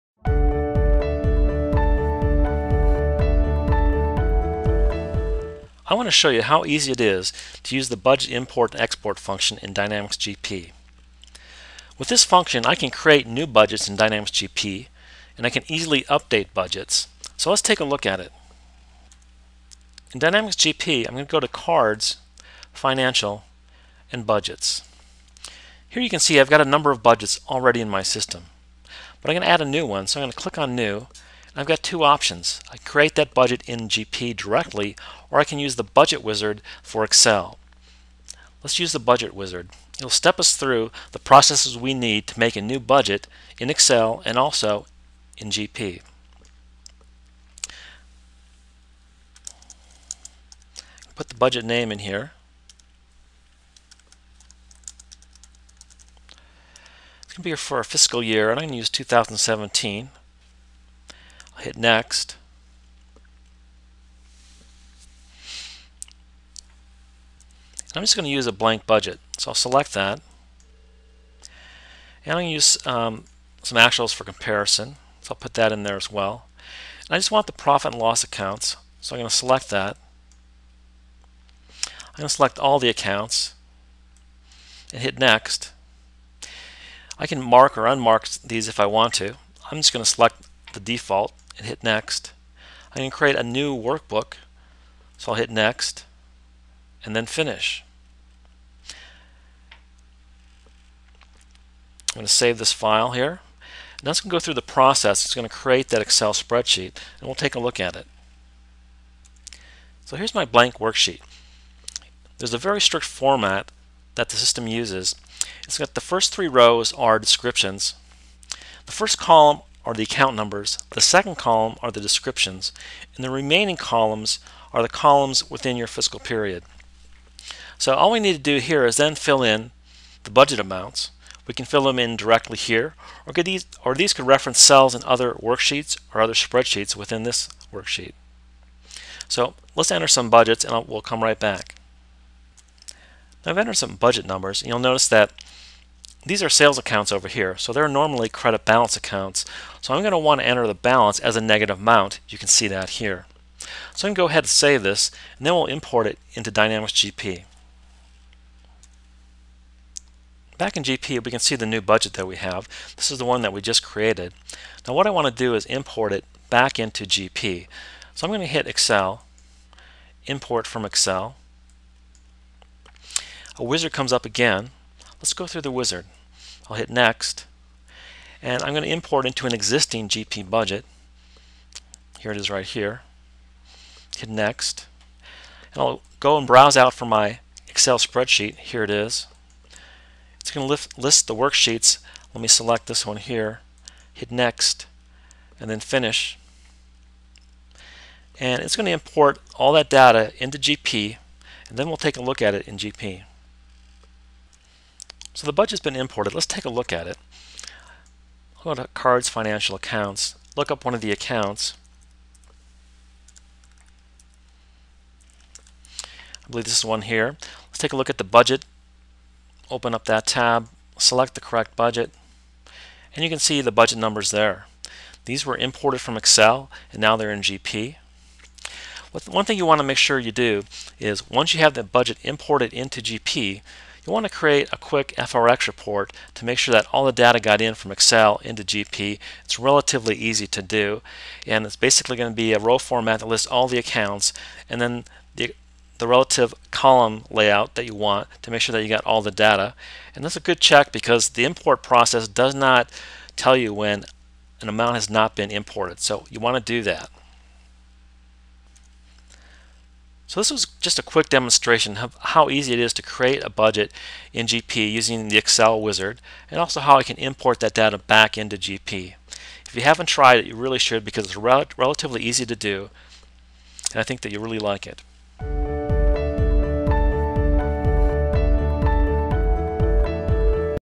Budget Import and Export Using Excel
Dynamics GP has a very easy to use budget import and export function that allows you to create budgets in Excel and update budgets in GP, based on amounts you have in Excel.